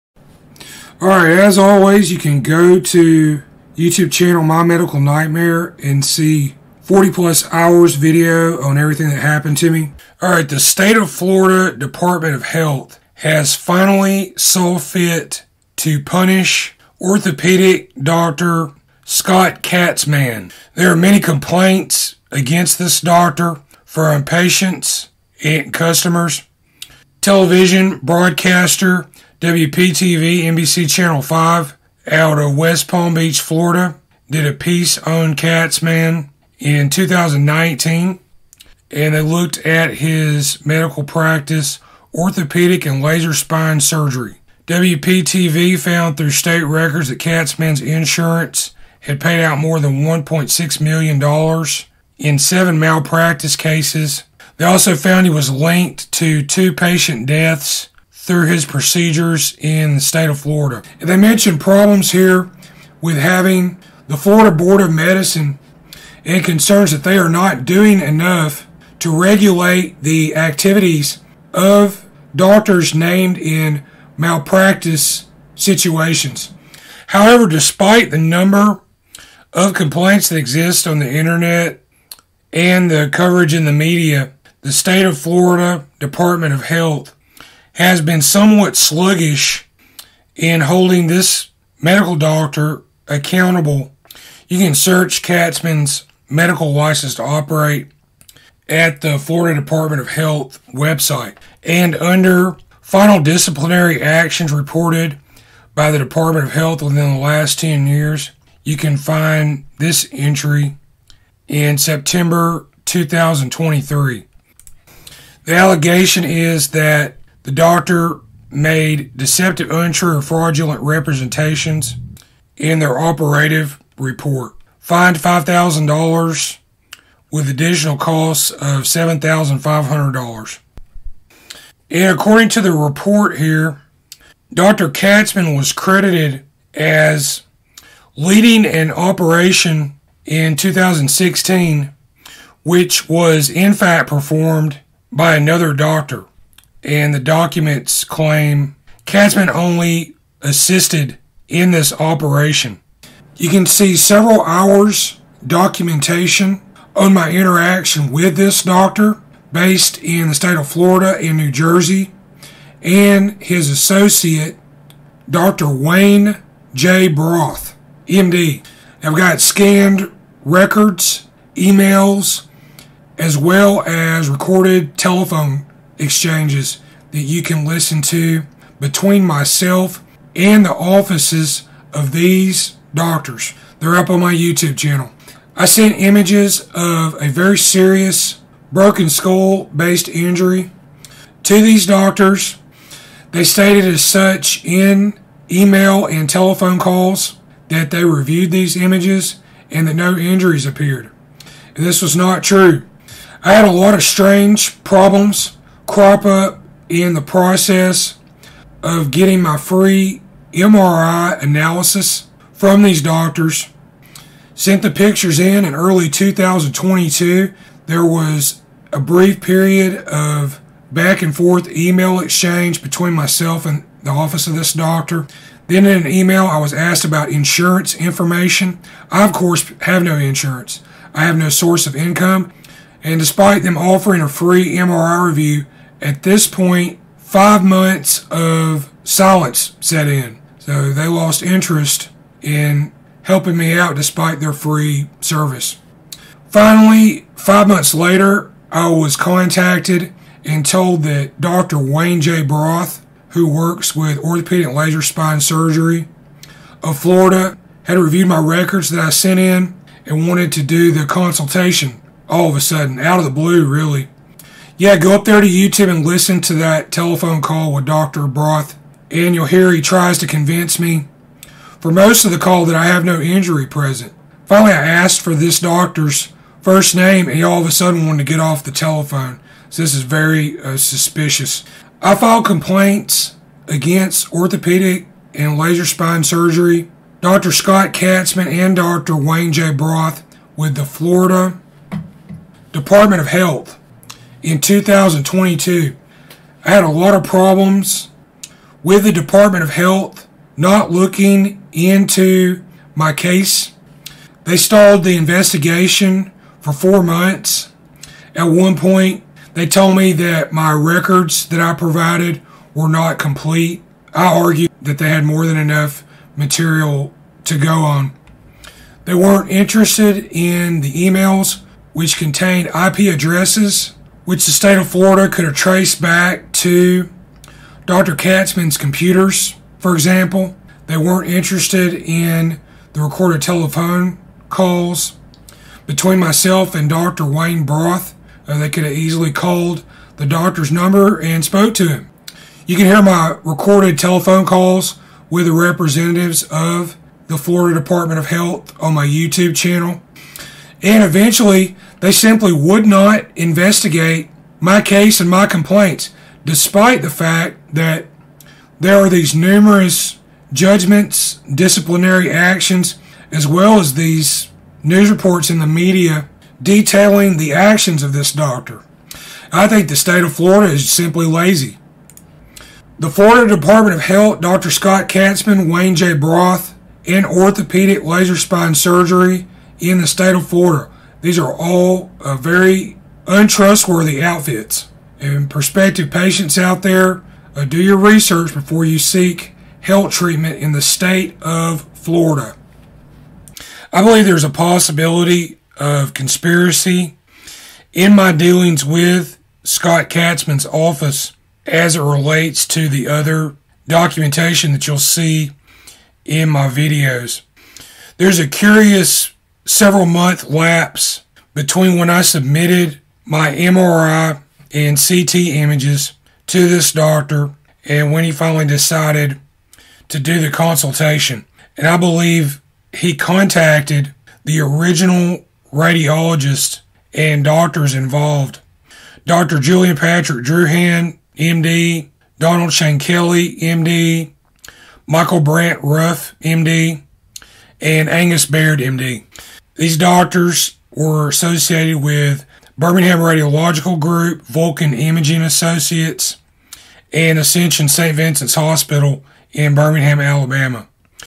Florida Dept. Of Health Finally Punishes MD Covered By TV News With Many Fraud Claims Outstanding
Florida State records indicate Doctor Scott Katzman of Orthopedic And Laser Spine Surgery of Florida was finally fined & disciplined for an offense in 2023 after many complaints, lawsuits by patients and customers. A TV news report in 2019 also pointed out that despite the public outcry & outstanding claims, the state was not doing enough to reprimand the MD and allowed him to continue to operate.

Television station WPTV NBC Channel 5 of West Palm Beach, Florida did a piece in 2019 which investigated Doctor Scott Katzman of Orthopedic And Laser Spine Surgery of Florida. WPTV found through state records, that Katzman's insurance has paid out more than $1.6 million involving seven malpractice cases and two patient deaths in Florida.

I had my own dealings with Dr. Katzman in 2022 in which he waited 5 months before appointing a junior MD to speak to me about my case and tried to convince me no injuries appeared in MRI which showed 6 inches diameter of infection surrounding a fracture in the bottom of my skull that had gone mistreated and unrecognized for about a year. When I asked the first name of Dr. Wayne J. Broth MD he refused to give it and abruptly ended our recorded telephone call (you can hear it in hours of videos documenting my dealings with these doctors). Strangely, the state of Florida refused to investigate my complaint regarding the practice of a several-times reprimanded doctor.

In August, 2022 I filed a complaint with The State Of Florida Department Of Health VS Dr. Wayne Broth of Florida and New Jersey.

Dr. Wayne Broth also called me from a 908 area code although a person licensed to practice medicine by that name does not show up on the New Jersey license look up page.

After nearly 5 months I am finally put in contact with Dr. Wayne J. Broth, OLSS spinal consultant. I sent my MRI images which were misread by other doctors to New Jersey & Florida based Dr. Scott Katzman's office (Orthopedic & Laser Spine Surgery). His website advertises a free MRI Review. I sent in images and records from many of my MRI and CT scans, showing a very serious head injury which has slowly healed from 2018. I had broken the underside of my skull and had parasites and infection develop over several months. These injuries are clearly visible in the images presented, not noted by the original reading radiologists. In March, 2022, after a request was sent to me for insurance information from past medical visits (I have no insurance), communication from this office ceased (I tried to call back, but was only able to leave a message on their machine).

Then in June I started receiving emails from Jenny, the office receptionist and once again called but was never contacted until July 29th. I was given a number to a Doctor Broth and recorded my telephone conversation with him which can be heard in part 2 of this video. Dr. Broth spends much of the video asking me repeatedly "what doctor told me I have an injury present". Real doctors practicing medicine don't ask such questions, they use their own experience to independently assess scientific and medical data. My experience is doctors who are trying to cover for another MD's error as such questions repeatedly. The broken bone injury, the infection injuries and 3 inch long insects that grew in the wound are clearly visible in the MRI and CT images I emailed them. They offer a service to independently review MRI images and my experience here is they did not give a correct analysis. When I simply asked his first name he abruptly wanted end our conversation, this is a huge red flag, and an indicator he knew he was giving me incorrect advice. This is unprofessional behavior for a licensed doctor to engage in.

I was denied due process by the State Of Florida trying to a file a complaint with merits against an offending doctor with a Florida medical license.